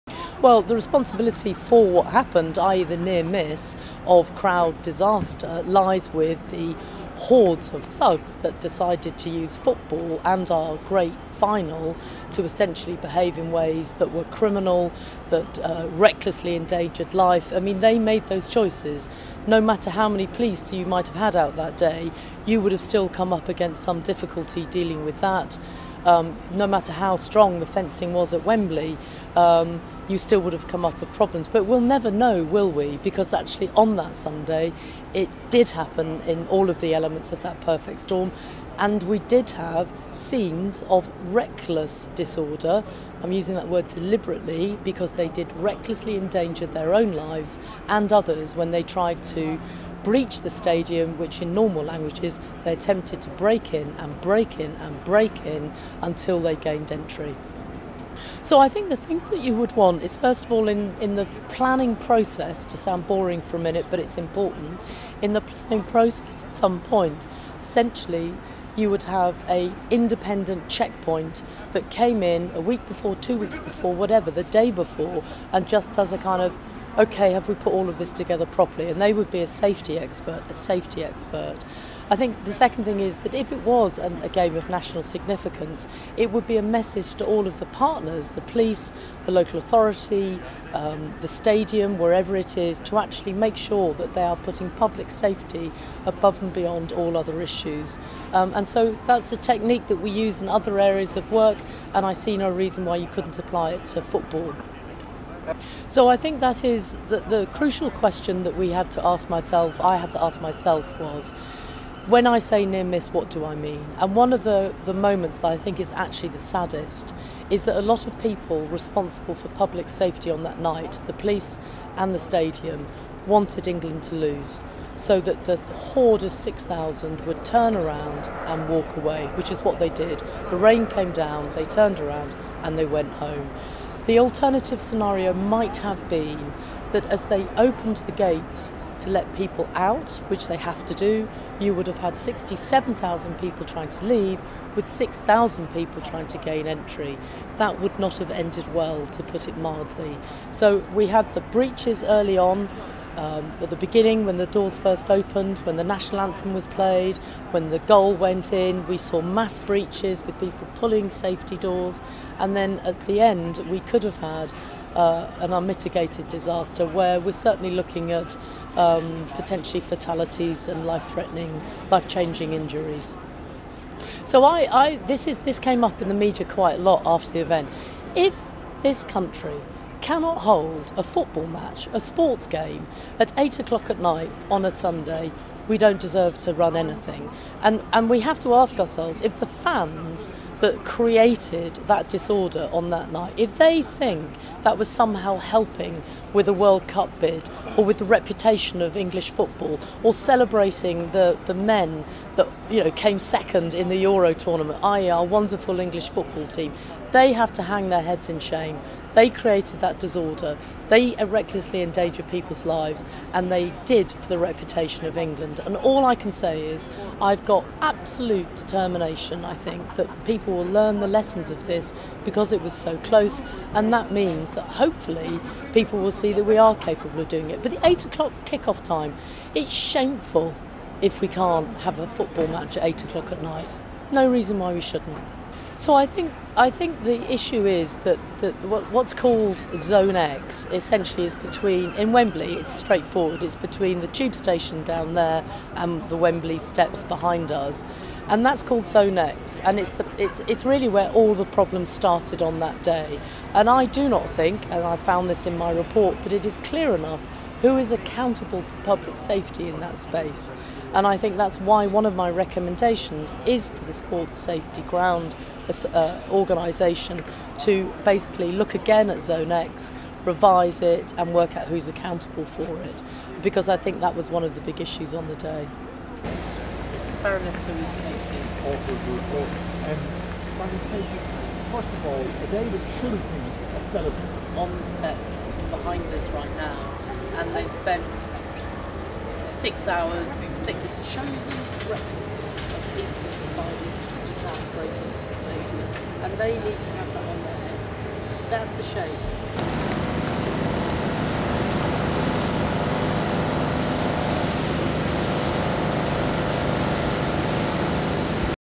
Baroness Casey comments on Euro 2020 Wembley disorder report
Baroness Casey comments on her independent review into events surrounding the chaotic scenes outside Wembley Stadium as England took on Italy in the Euro 2020 final on July 11, which saw around 2,000 people without tickets manage to get into the ground amid scenes of mass disorder.